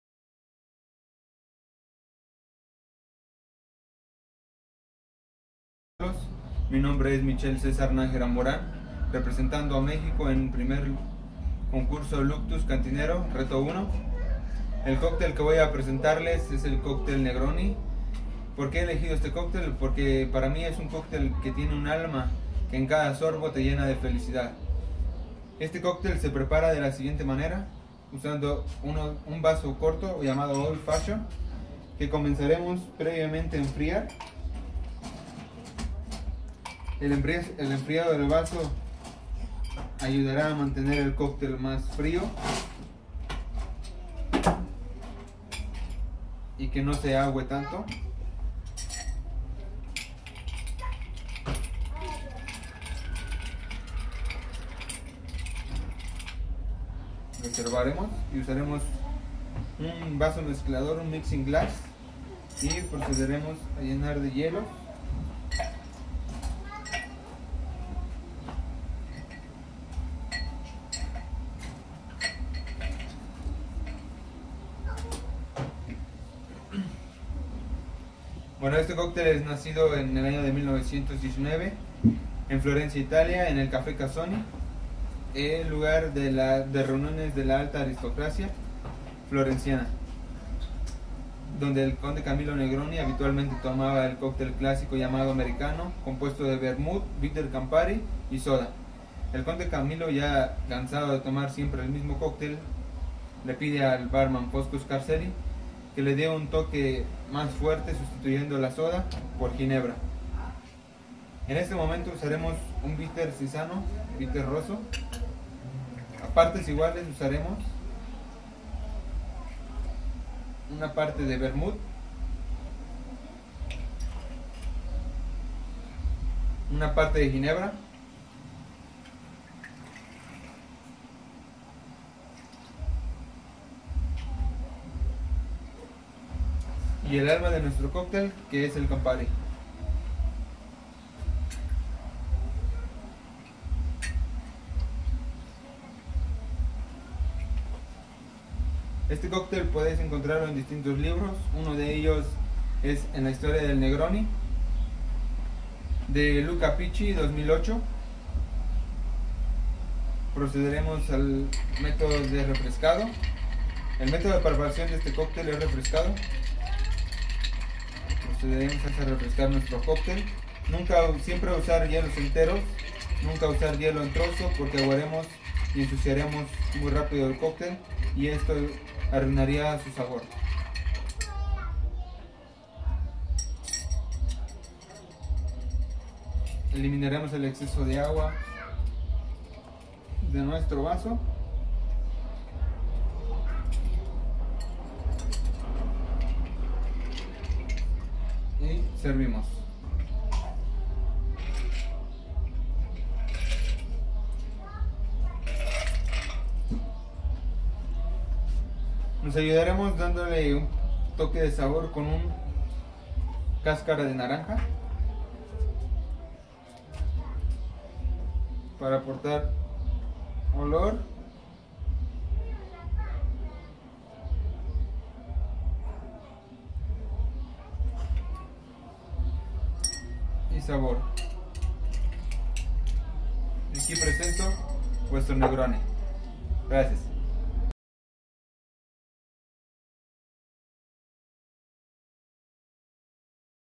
MICHEL NAJERA - MEXICO, PREPARA UN NEGRONI
MICHEL NAJERA acepto el primer reto y compite en Luctus Cantinero, competencia online de La Guia del Cantinero representando a su hermoso pais MEXICO.
Ayudalo a ganar con tu voto, (VOTOS VALIDOS SOLO EN YOUTUBE o en la pagina de facebook La Guia del Cantinero) solo dale me gusta al vídeo, si quieres dejale un comentario y sobre todo comparte este video para que tus amigos tambien voten por tu favorito.

Nos presento su receta del Negroni
1 parte de Vermouth rosso
1 parte de Gin
1 parte de Campari

Metodo de elaboracion: refrescado
Servido en vaso old fashion con hielo y aromatizado con piel de naranja